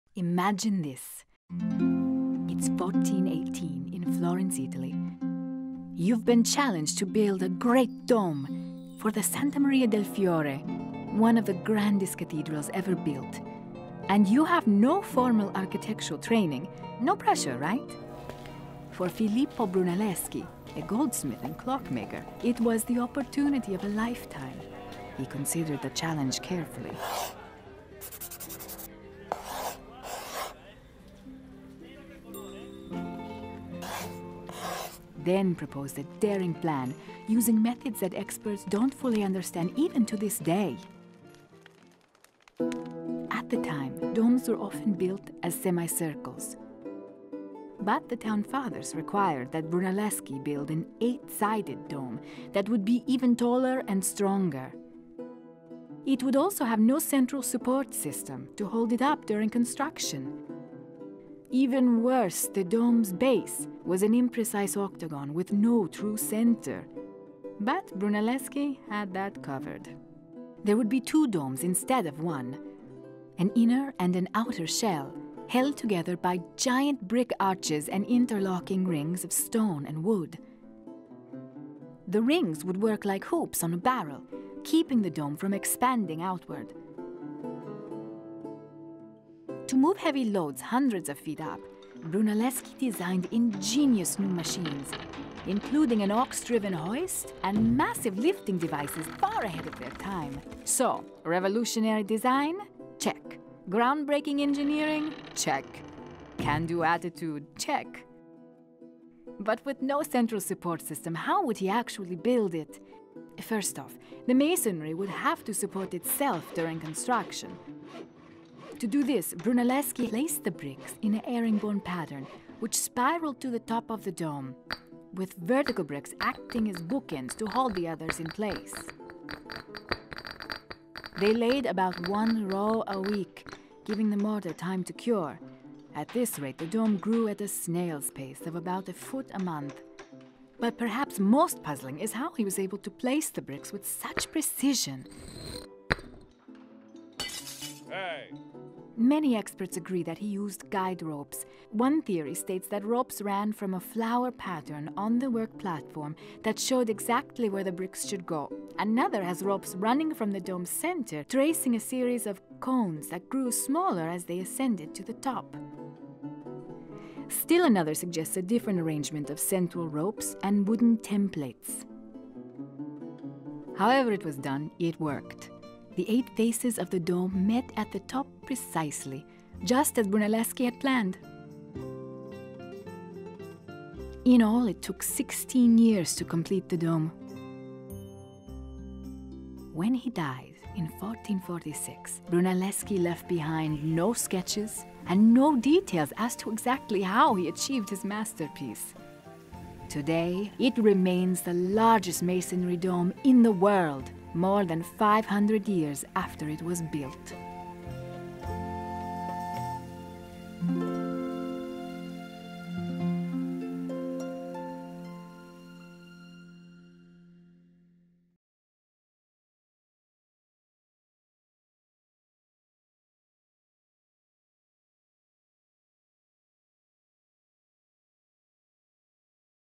How an Amateur Built the World's Biggest Dome
In 1418, Filippo Brunelleschi was tasked with building the largest dome ever seen at the time. He had no formal architecture training. Yet experts still don't fully understand the brilliant methods he used in contructing the dome, which tops the Santa Maria del Fiore cathedral in Florence, Italy.

PRODUCER, EDITOR, AND WRITER: Hans Weise
ART DIRECTOR: Fernando G. Baptista
ART AND ANIMATION: Fernando G. Baptista and Matthew Twombly
MAP AND TYPOGRAPHY: Lauren E. James
ADDITIONAL WRITING: Jason Orfanon
NARRATOR: Paula Rich
RESEARCH: Fanna Gebreyesus and Elizabeth Snodgrass
SPECIAL THANKS: Riccardo Dalla Negra, Massimo Ricci, and Francesco Gurrieri